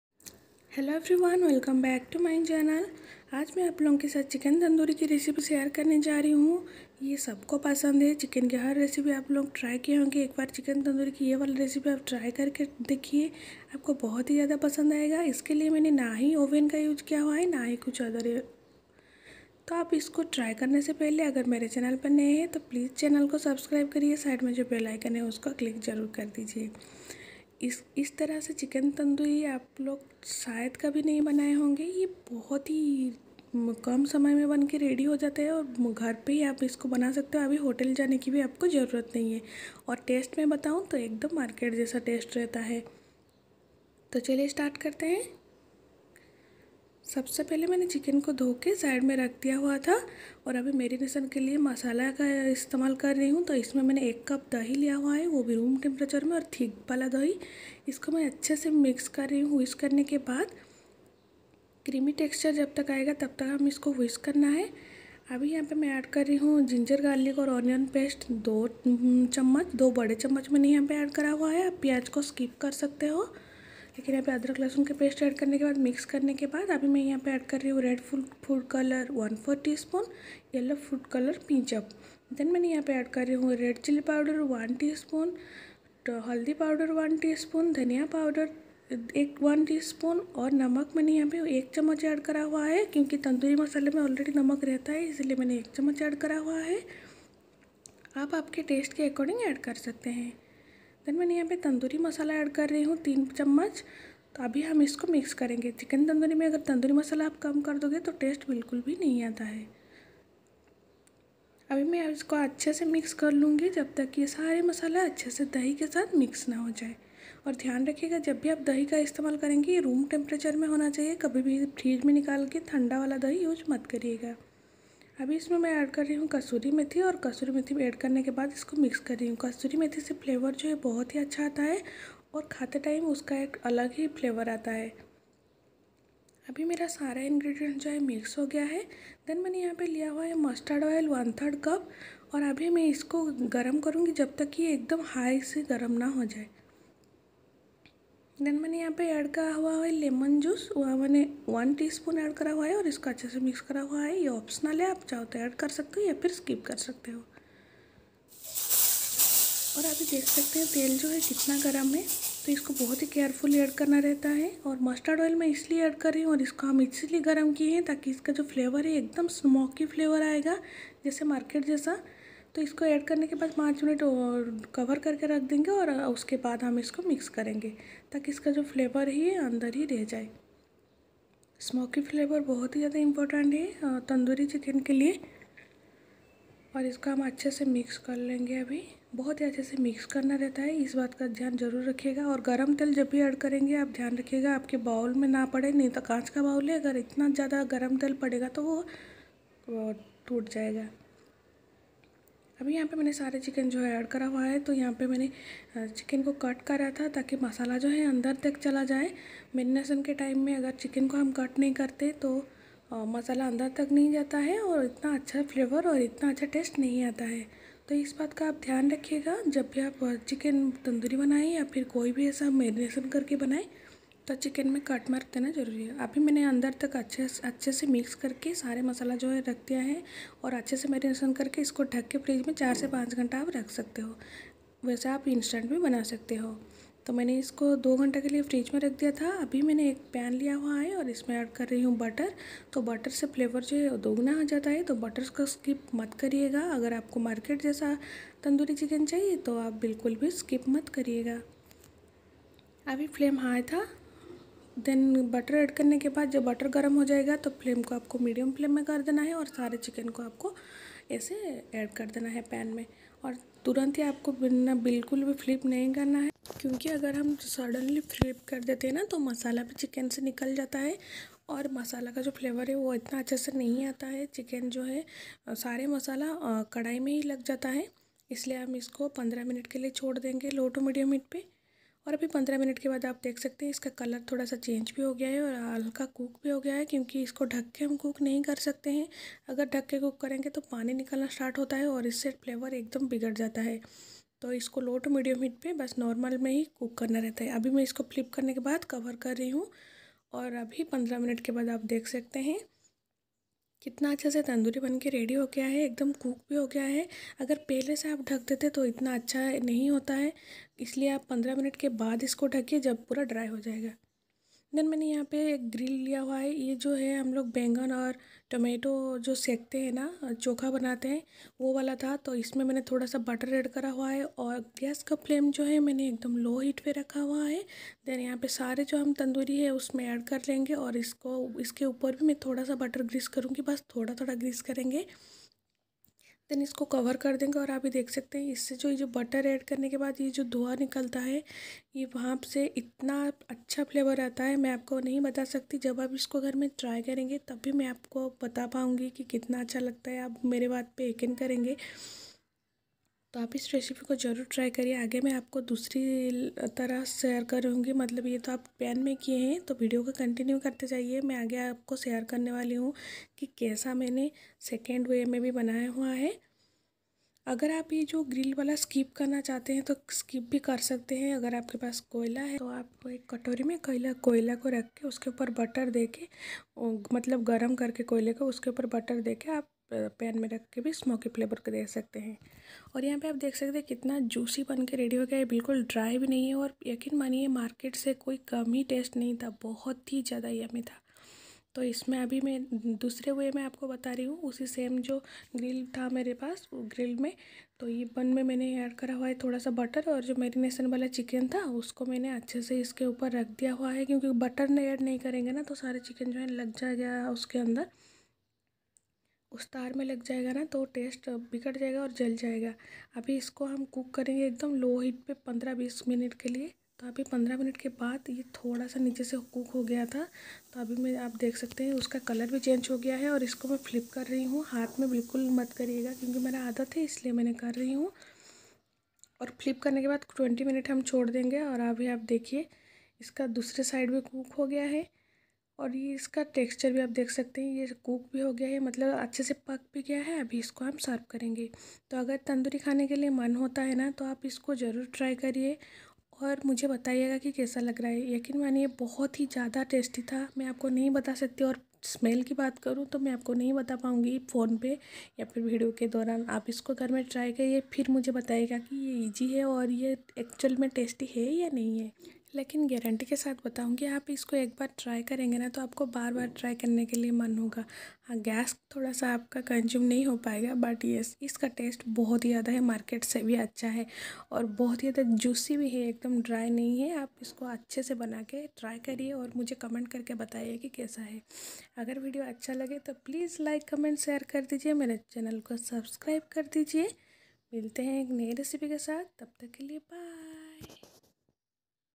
Tandoori chicken recipe | No oven Tandoori chicken recipe | How to make Grilled chicken at home
ingredients :--

ginger+garlic+onion paste - 2 tbsp
salt to taste - 1 TSP
tandoori masala - 3 TSP
red food colour - 1/4 TSP
yellow food colour - pinch of